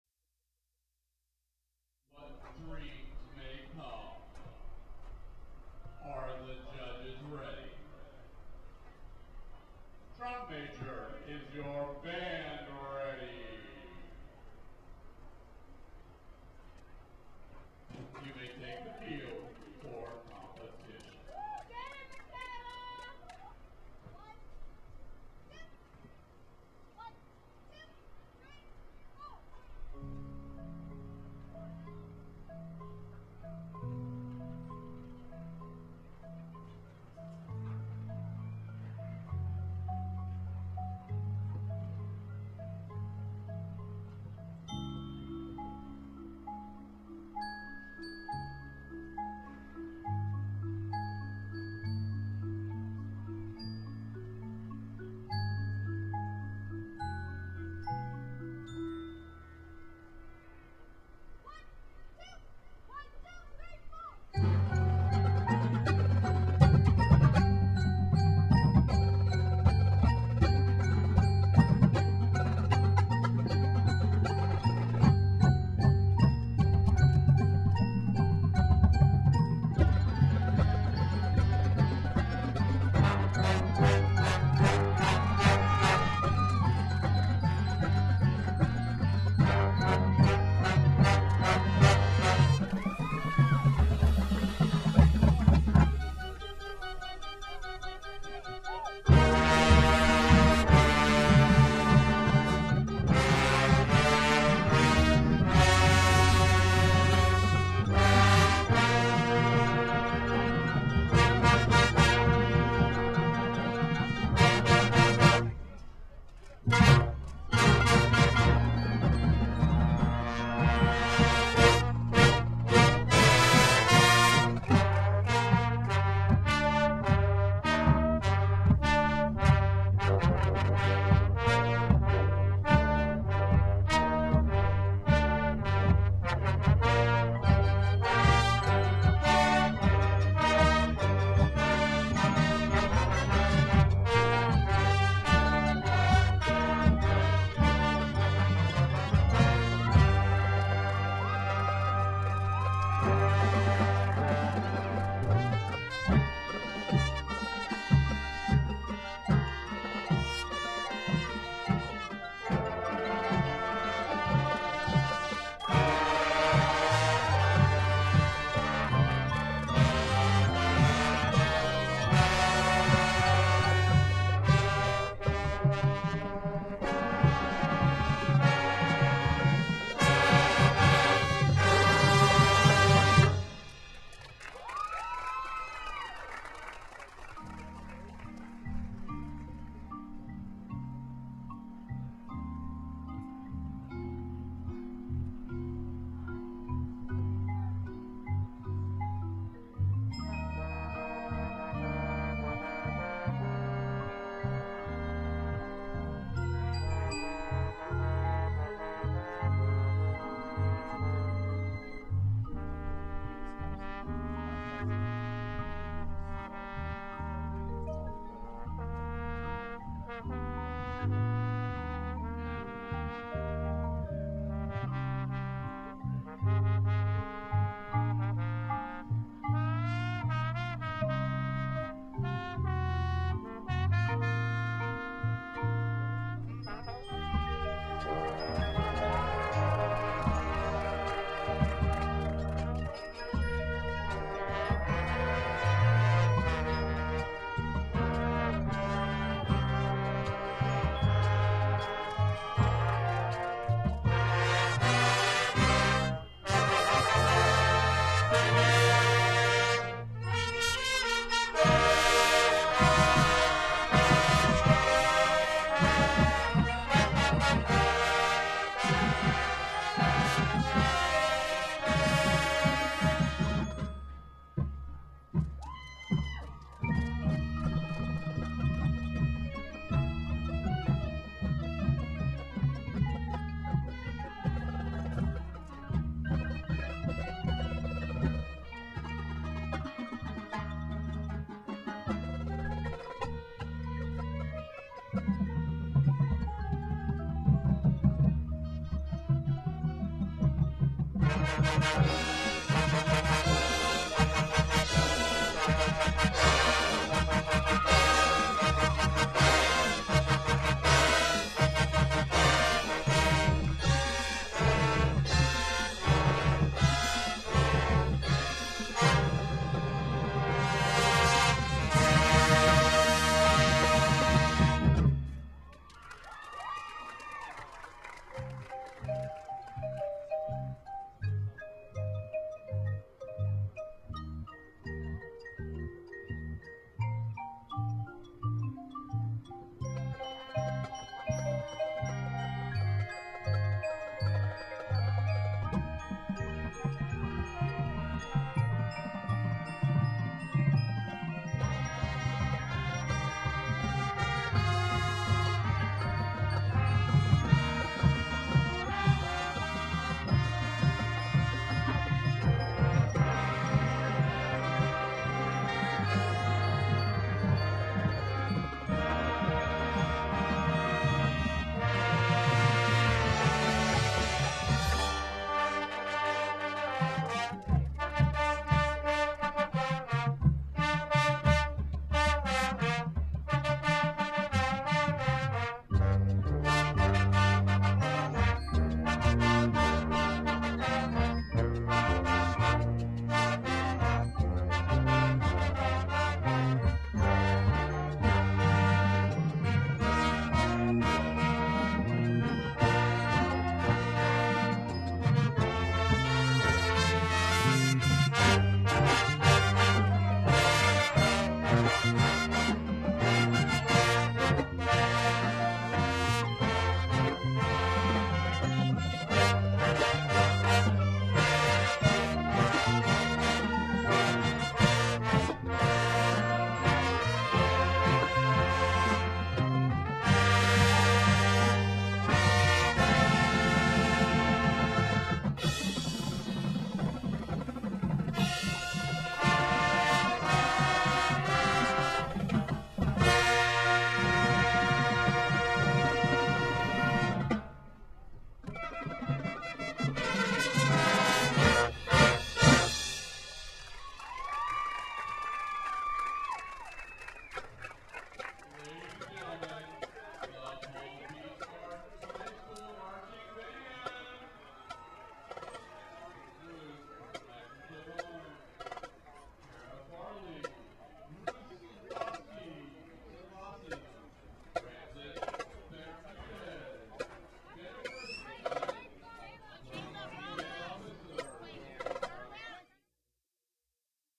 Palm Beach Gardens
Palm Beach Gardens High School Marching Band from the 2013 Park Vista Striking Cobra Invitational. Produced by BandLife.com